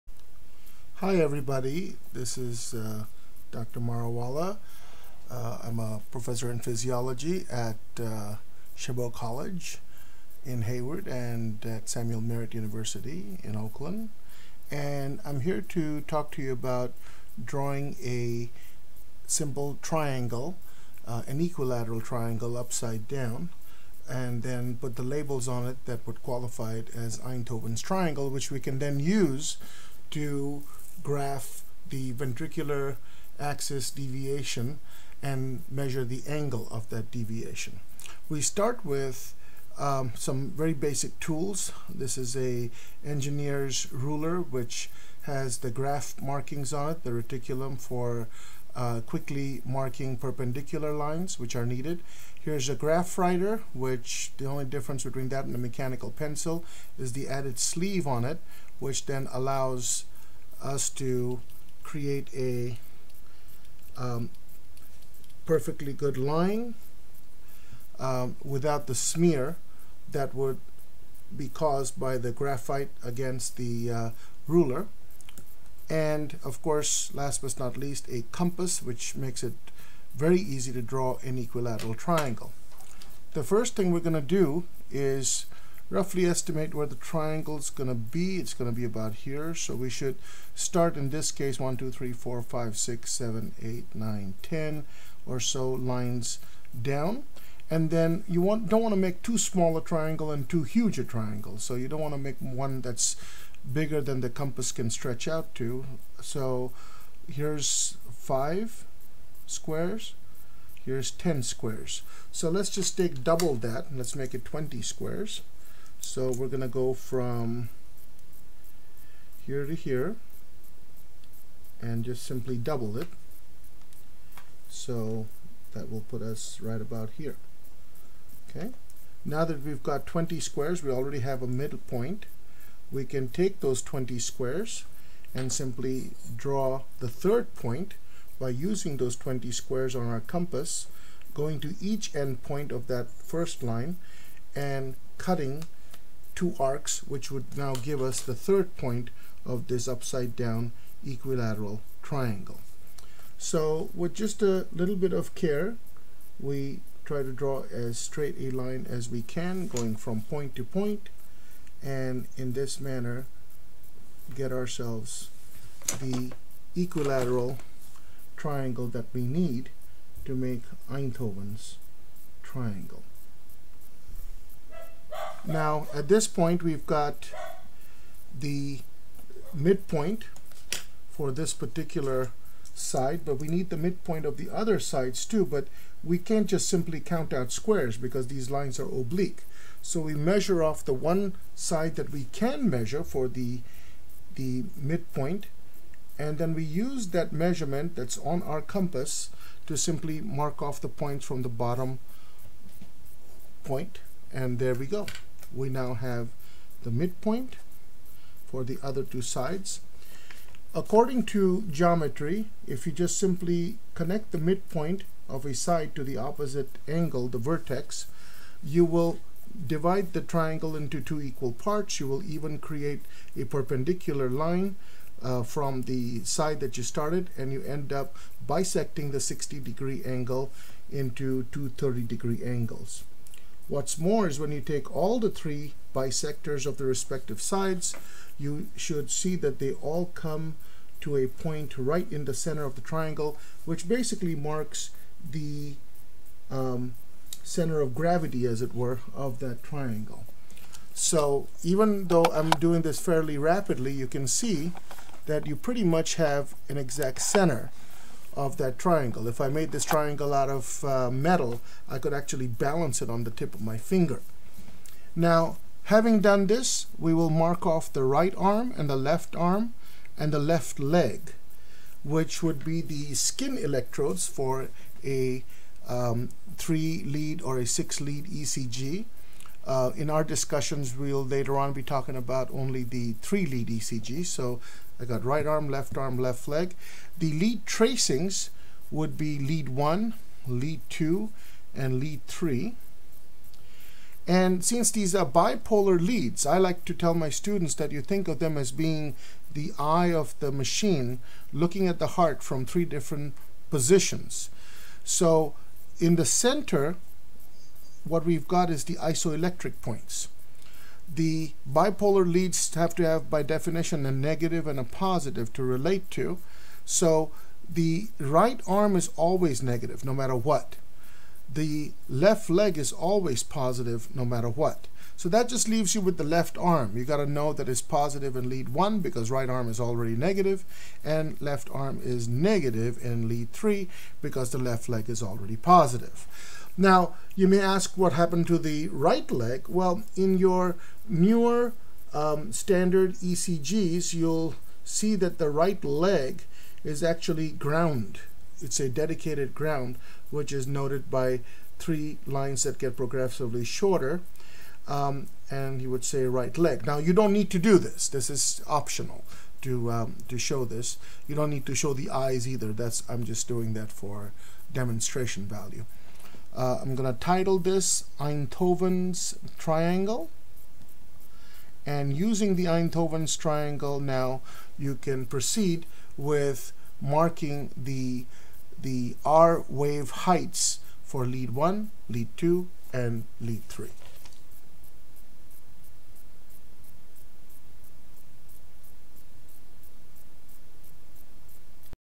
Drawing Einthoven's triangle
Instructions to draw an Einthoven's triangle for Physiology students so that they can graph the heights of the R waves in leads I, II, III.
The RL does not have to be included in the drawing but it is a dedicated grounding lead.